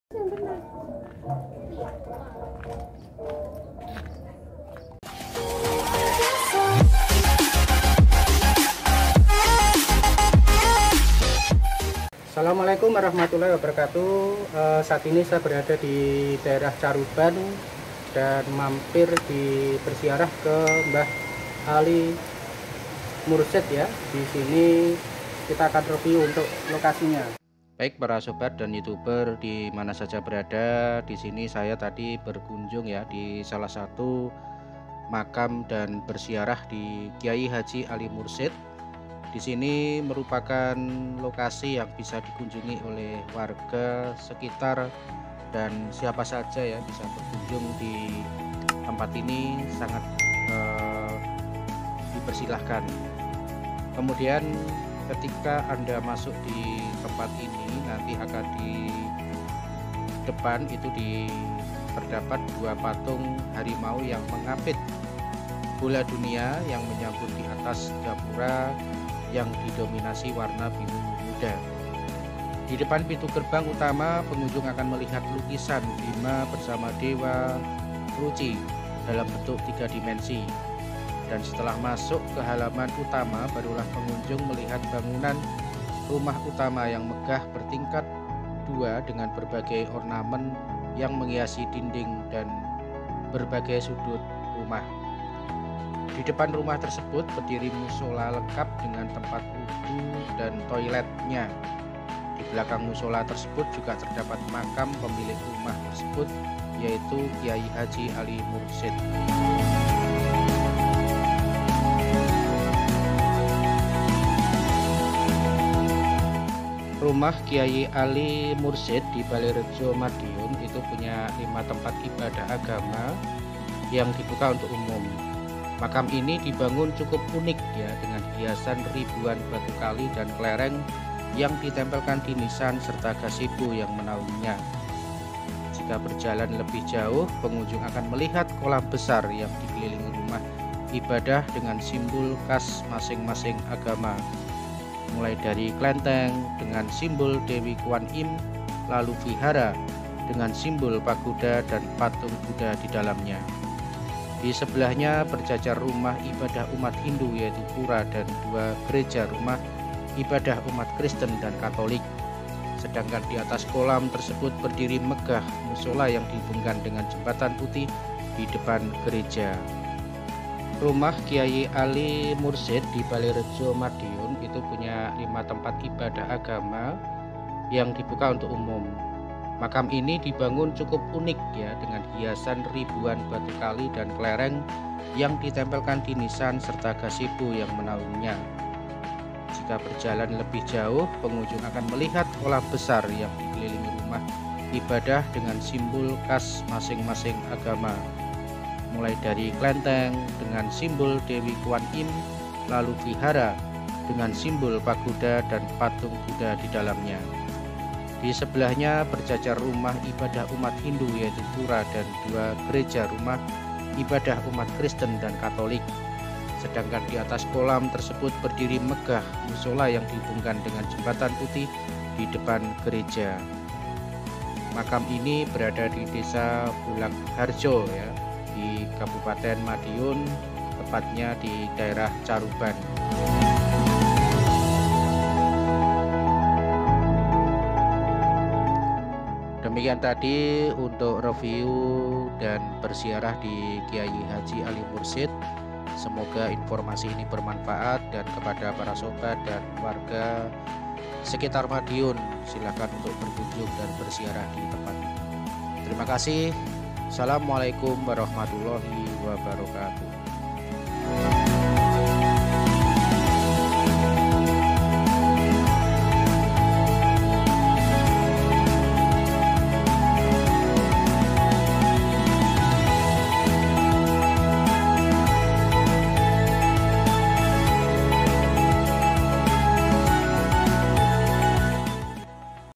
WISATA RELIGI | Makam Rumah Megah Alm. Ali Mursyid Balerejo Madiun
Sebuah rumah megah berdiri di Desa Bulakrejo, Balerejo, Kabupaten Madiun.
Berbeda dengan rumah yang lain, rumah ini mempunyai tempat ibadah dari lima agama, mulai dari Mushola, Klenteng, Vihara, Pura, hingga Gereja.
Rumah tersebut dibuka untuk umum.